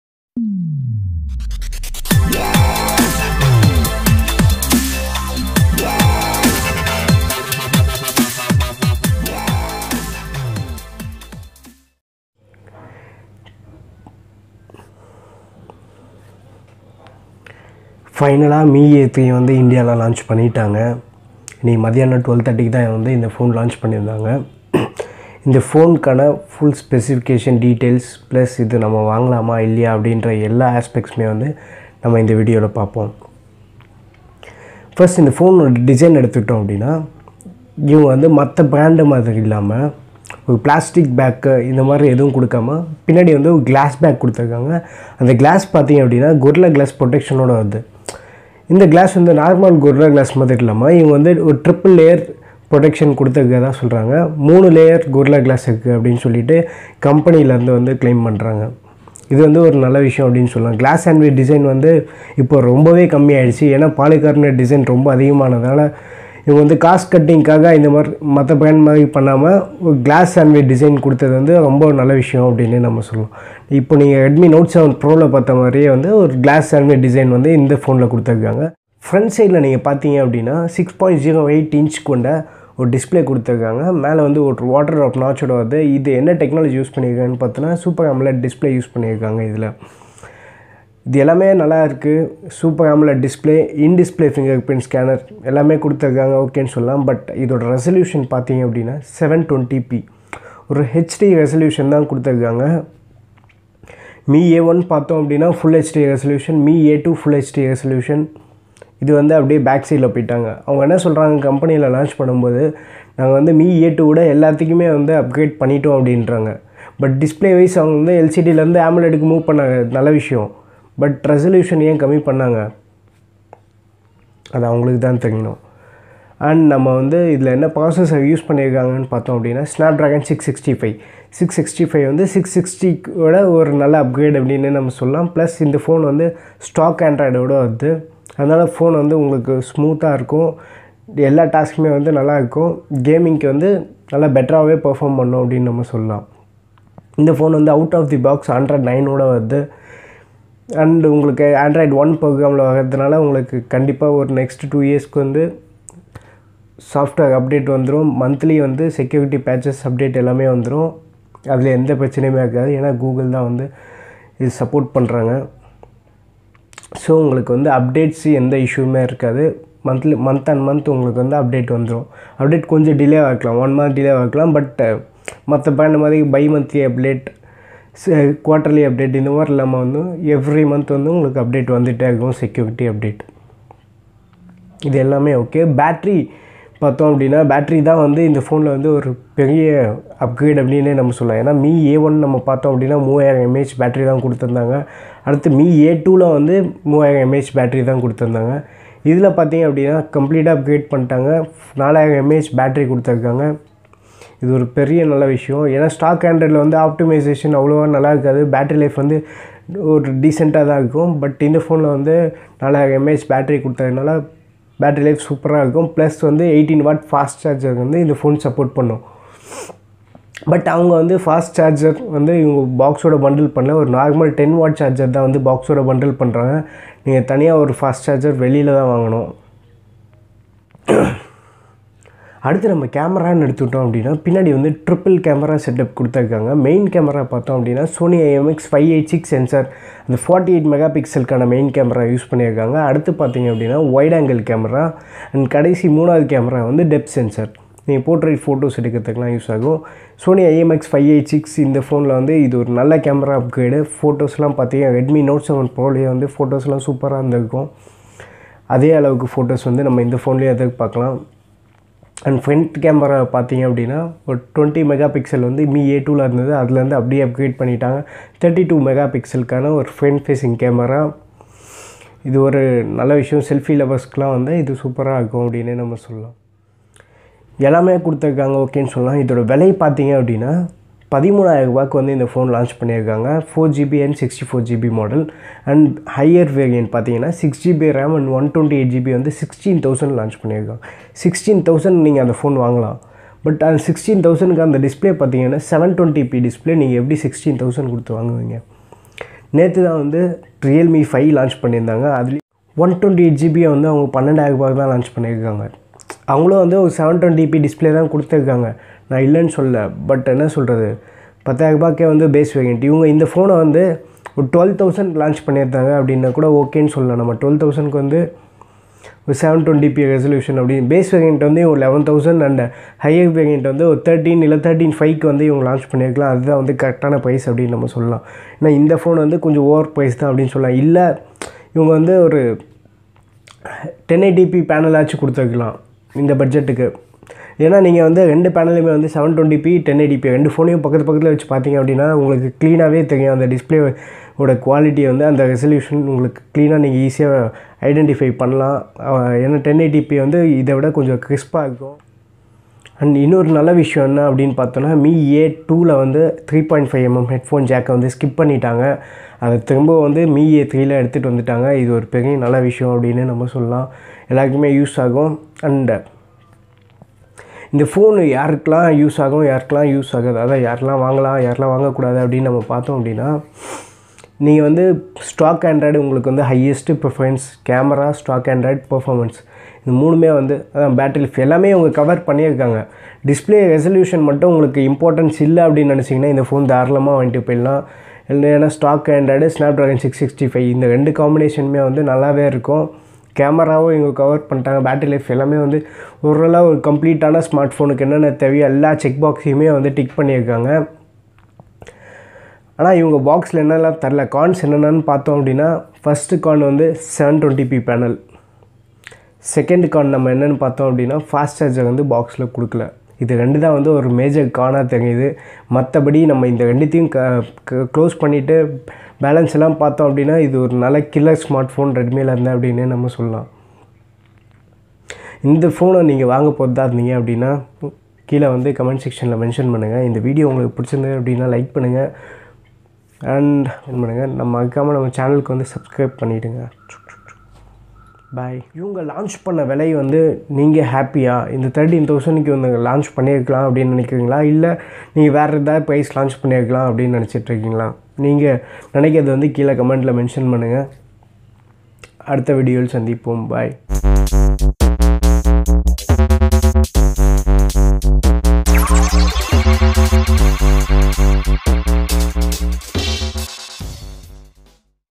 Mi A3 in Tamil 32MP front camera, Snapdragon 665 , 4000mah battery
Xiaomi has launched the Mi A3 in India .
4/64 @12999
6/128 @15999
Device is available in Amazon.
first sale from 23rd August

free to share your opinions in the comments section below .